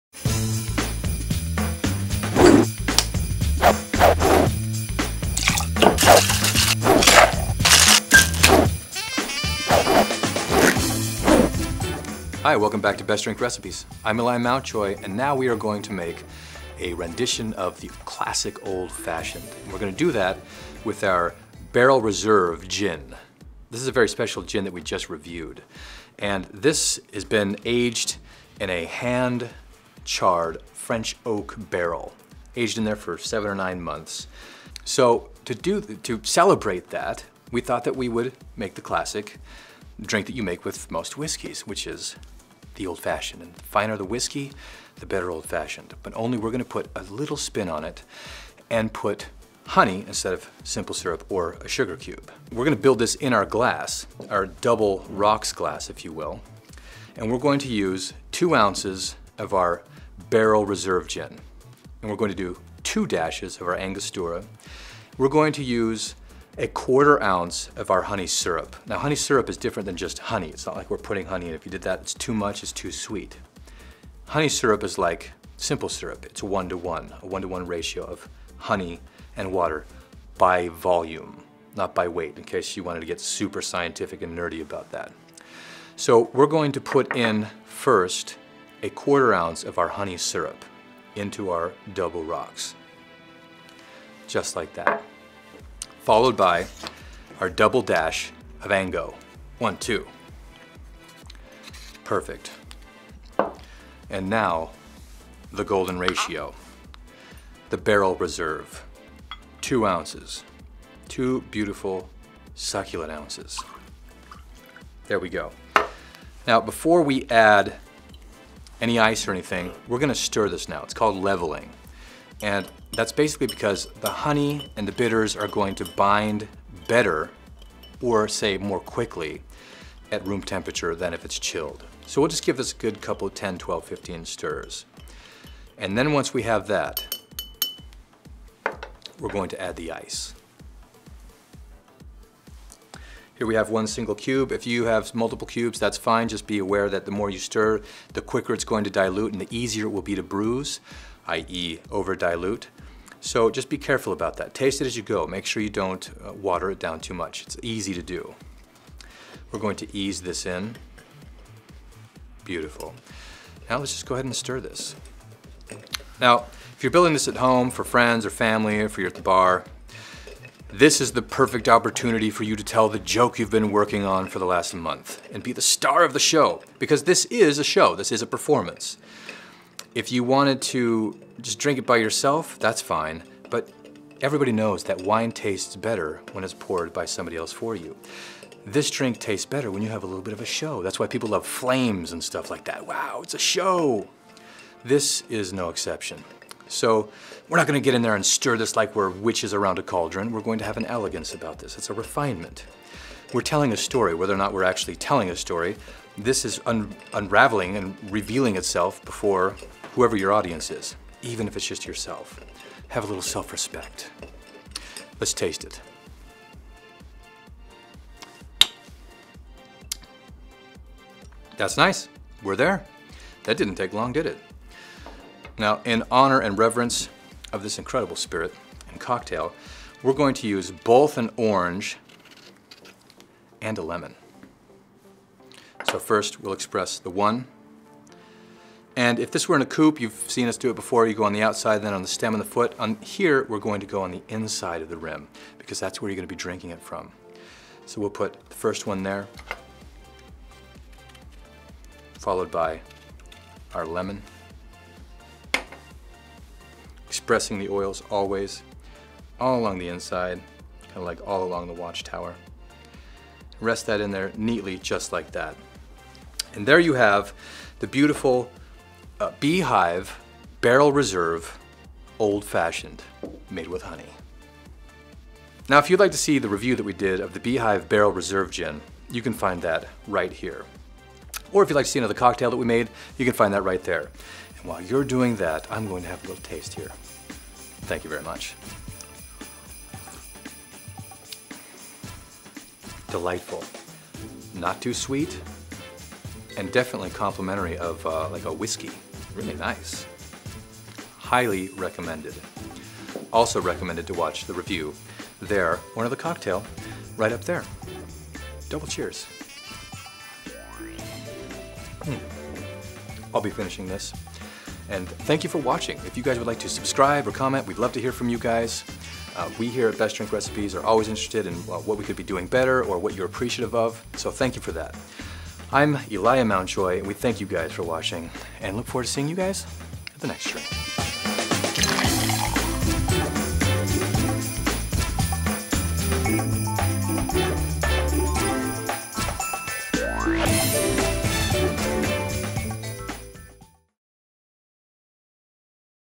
How to Make The Barrel Reserve Gin Old Fashioned - Featuring Beehive Barrel Reserve Gin
The Old Fashioned is a cocktail made by muddling sugar with bitters, then adding alcohol, such as whiskey or brandy, and a twist of citrus rind. It is traditionally served in a short, round, 8–12 US fl oz (240–350 ml) tumbler-like glass, which is called an Old Fashioned glass, named after the drink.

The Old Fashioned, developed during the 19th century and given its name in the 1880s, is an IBA Official Cocktail. It is also one of six basic drinks listed in David A. Embury's The Fine Art of Mixing Drinks.

The first documented definition of the word "cocktail" was in response to a reader's letter asking to define the word in the May 6, 1806, issue of The Balance and Columbian Repository in Hudson, New York. In the May 13, 1806, issue, the paper's editor wrote that it was a potent concoction of spirits, bitters, water, and sugar; it was also referred to at the time as a bittered sling. J.E. Alexander describes the cocktail similarly in 1833, as he encountered it in New York City, as being rum, gin, or brandy, significant water, bitters, and sugar, though he includes a nutmeg garnish as well.

By the 1860s, it was common for orange curaçao, absinthe, and other liqueurs to be added to the cocktail. The original concoction, albeit in different proportions, came back into vogue, and was referred to as "old-fashioned". The most popular of the in-vogue "old-fashioned" cocktails were made with whiskey, according to a Chicago barman, quoted in The Chicago Daily Tribune in 1882, with rye being more popular than Bourbon. The recipe he describes is a similar combination of spirits, bitters, water and sugar of seventy-six years earlier.

Traditionally, the first use of the name "Old Fashioned" for a Bourbon whiskey cocktail was said to have been, anachronistically, at the Pendennis Club, a gentlemen's club founded in 1881 in Louisville, Kentucky. The recipe was said to have been invented by a bartender at that club in honor of Colonel James E. Pepper, a prominent bourbon distiller, who brought it to the Waldorf-Astoria Hotel bar in New York City.